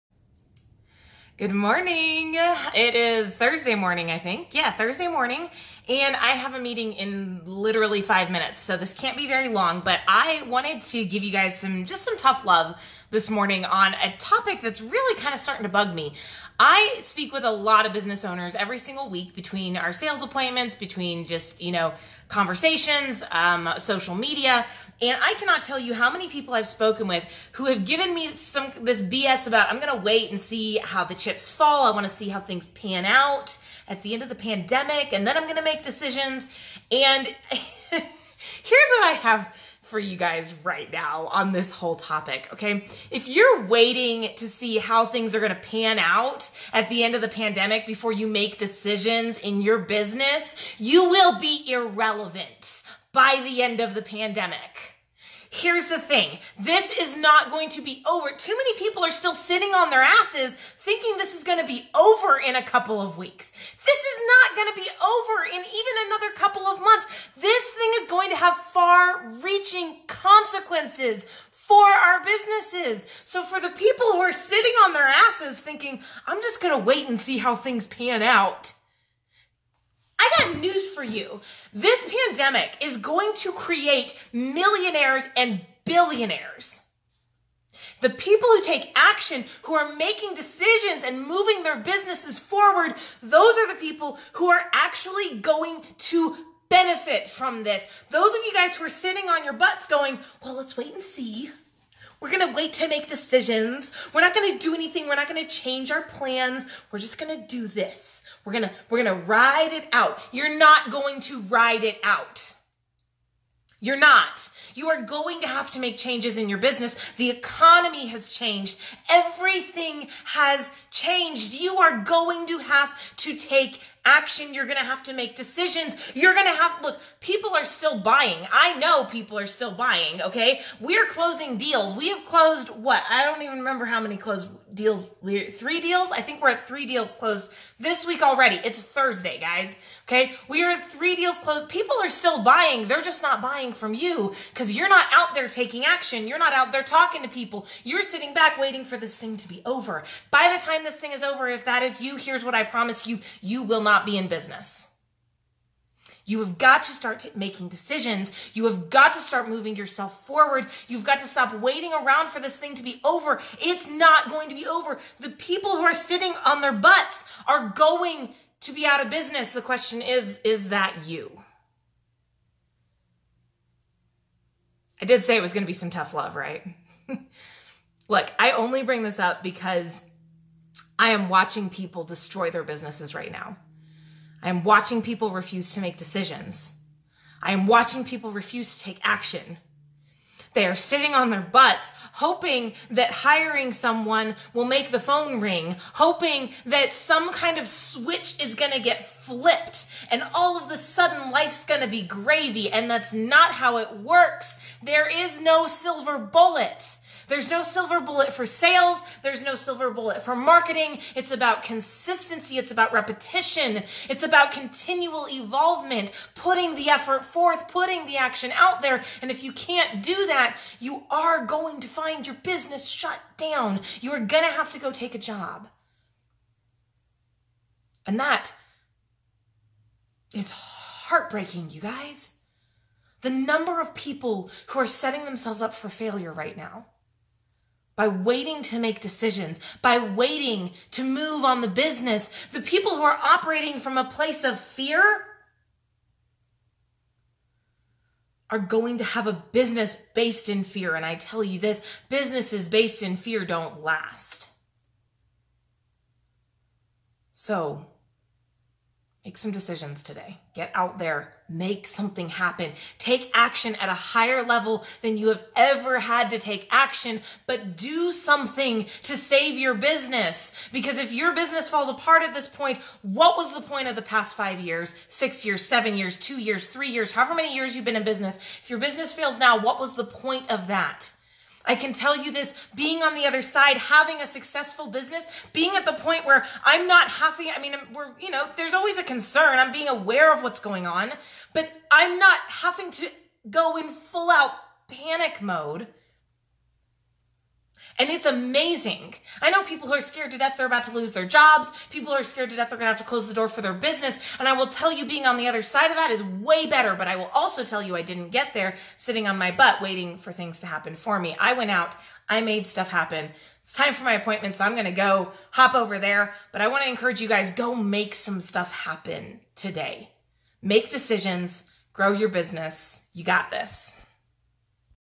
"Can I Give you Some Tough Love?" Award Winning Entrepreneur Shares Pandemic Business Tips
Stephanie sat down to share some tough love for small business owners who want to come out of this pandemic stronger!